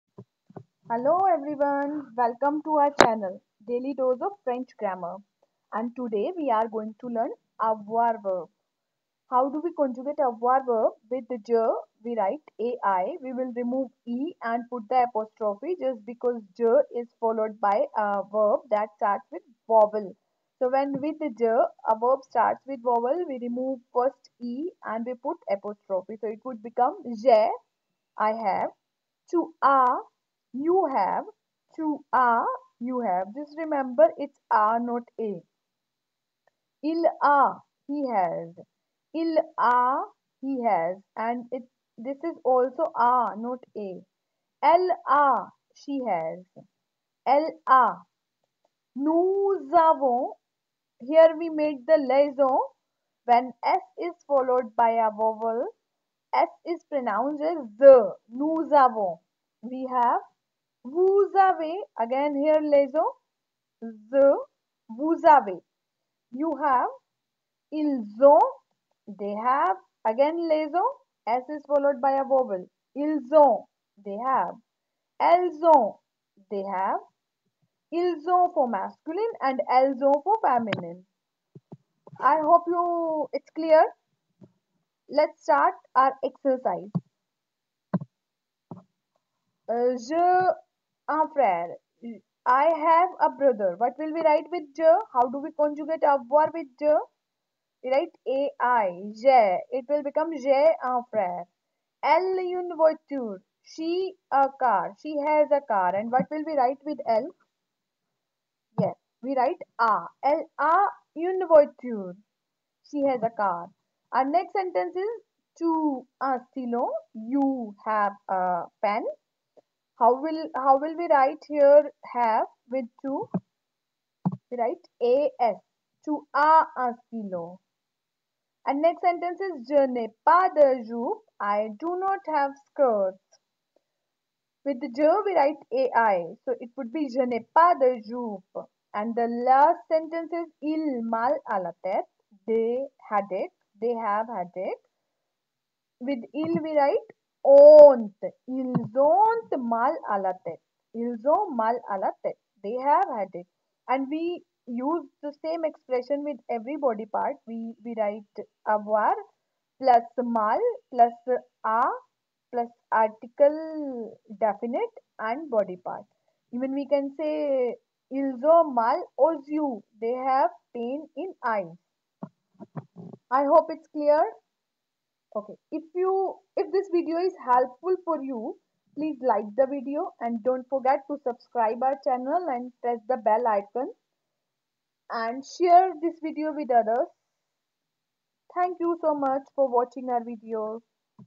Avoir verb ( has, have ): verb conjugaison and exercise
learn avoir verb in simplest way,  La conjugation du verb Avoir et une eercise
avoir - present de l'indicatif
J'ai
tu as
il, elle a
nous avons
vous avez
ils, elles ont